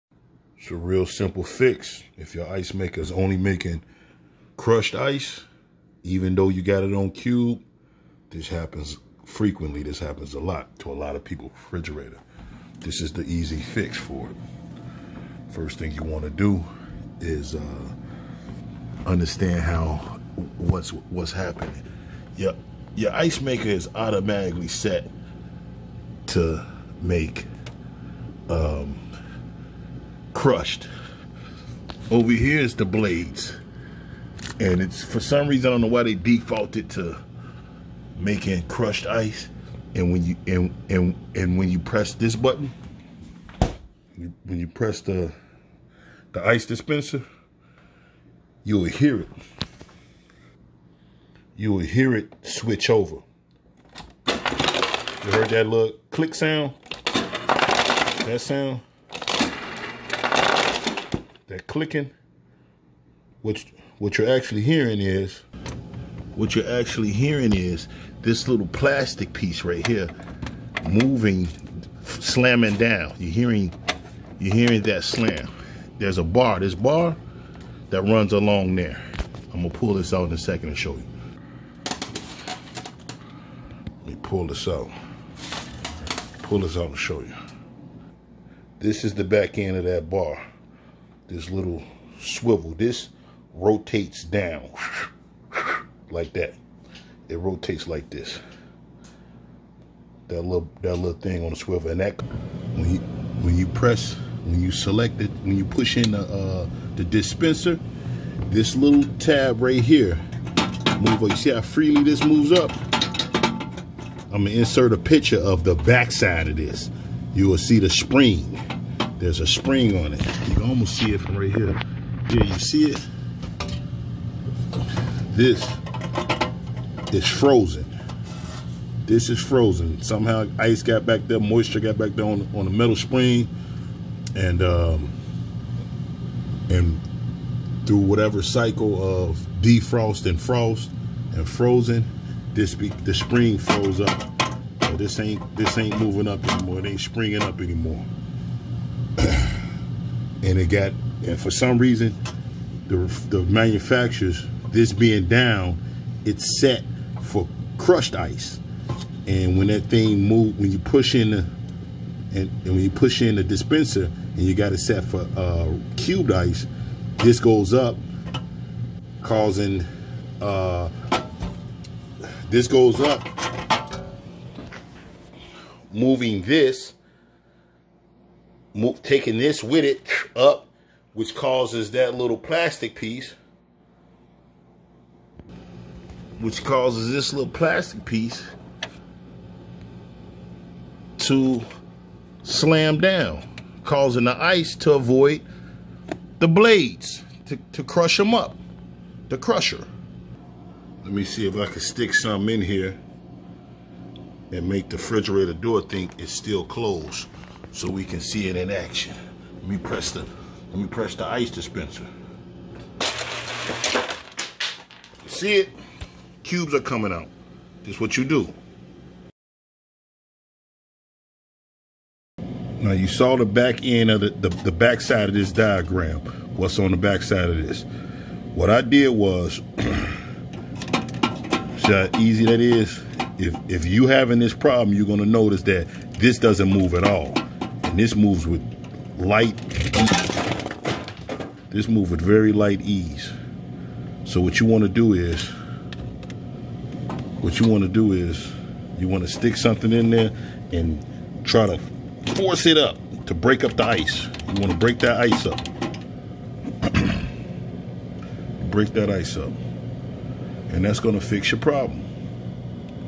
fix icemaker only making crushed ice. super easy
this is for icemakers that have a crushed option